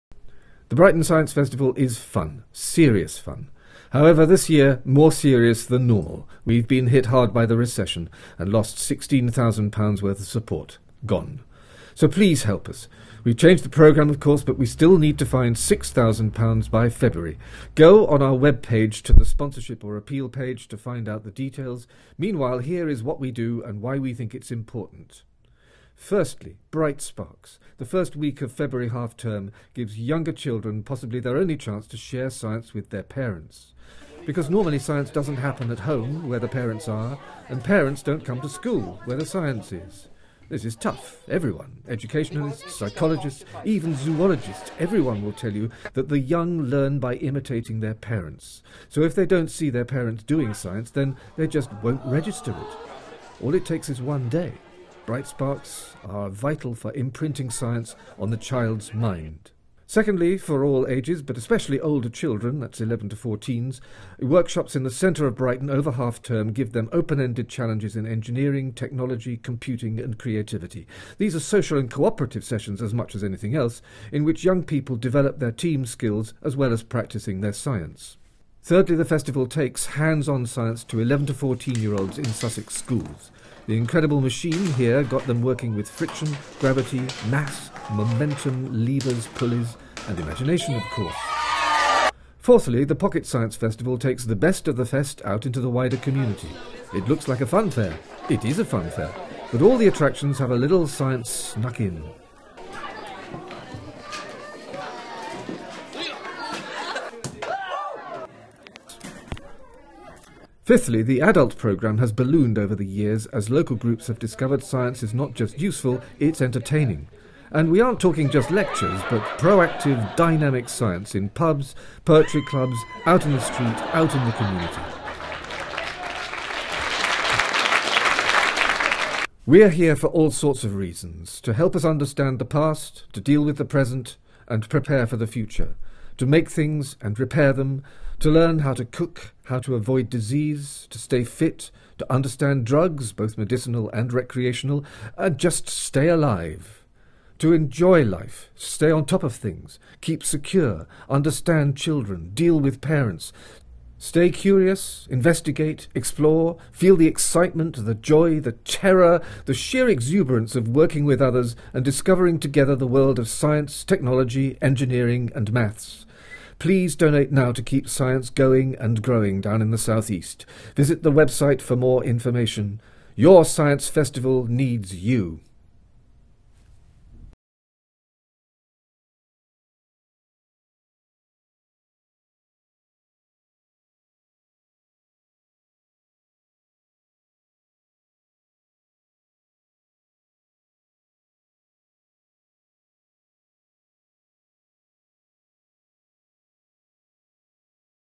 Festival Appeal
Your Science Festival needs YOU!

Special Festival Appeal to help us continue bringing the world's best science to Brighton and Brighton's best science to the world.

or click on the link at the end of the video to donate.

With thanks to Edward Cave, Francesca Moore, Colleen Slater, Jack Beard and Jon Simmons.